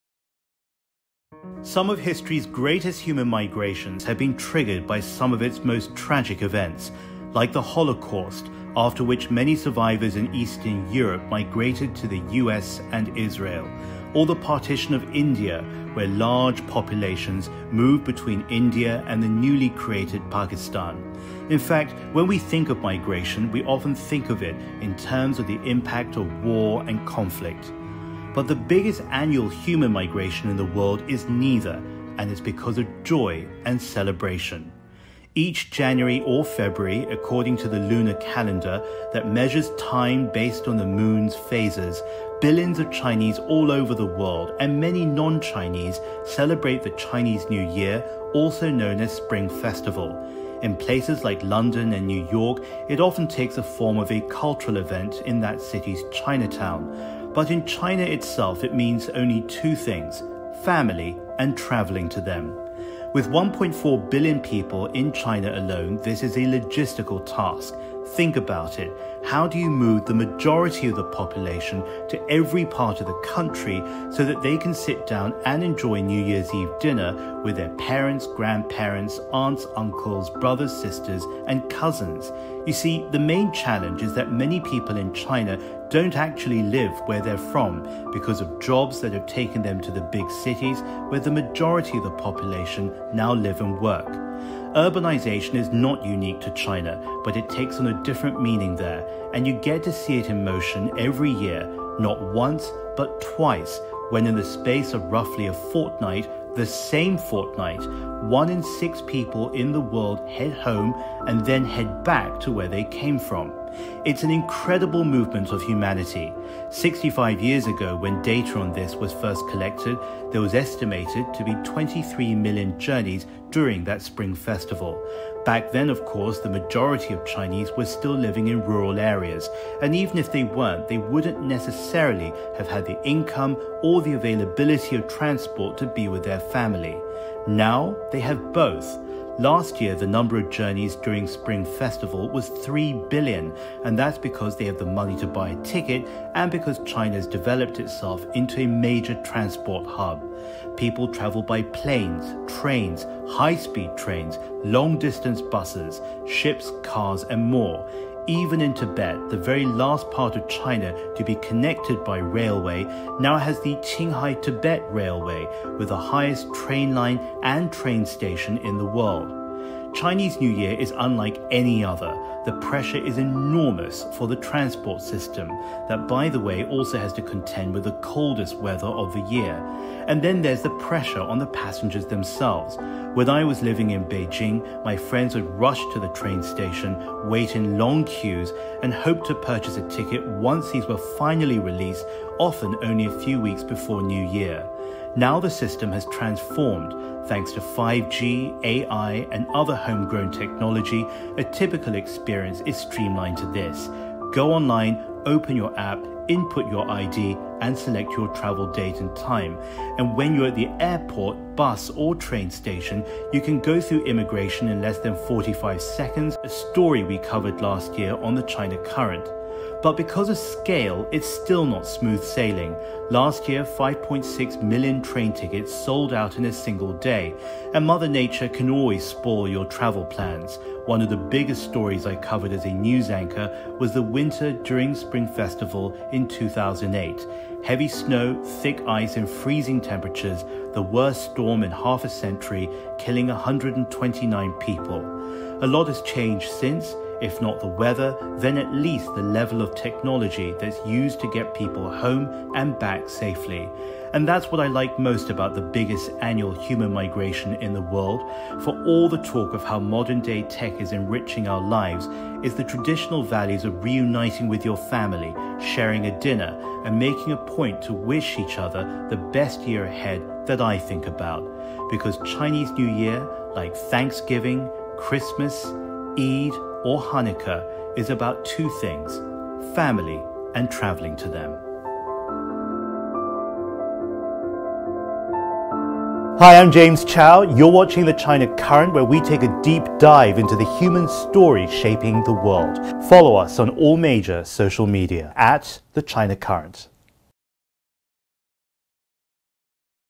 Going Home for the Holidays | Chinese New Year
The largest annual human migration is about to take place — 1 in 6 people in the world will be traveling home for Chinese New Year. Often, human migration is linked to tragedy and conflict, but this migration is because of joy and family. Similar to Christmas, Eid, or Hanukkah, Chinese New Year is traditionally a family holiday. Around the world, Chinese New Year is celebrated with cultural festivals and meals, often in local Chinatowns. In China, where most do not live in their hometowns, the holiday entices hundreds of millions to travel home. In 2019 alone, some 3 billion trips were made by plane, train, bus, or car — with 5.6 million train tickets bought in just a single day. Check out how modern transportation, AI, and 5G technology help China manage this immense logistical feat with modern ingenuity. As Chinese New Year approaches, we wish everyone safe travels and a happy holiday!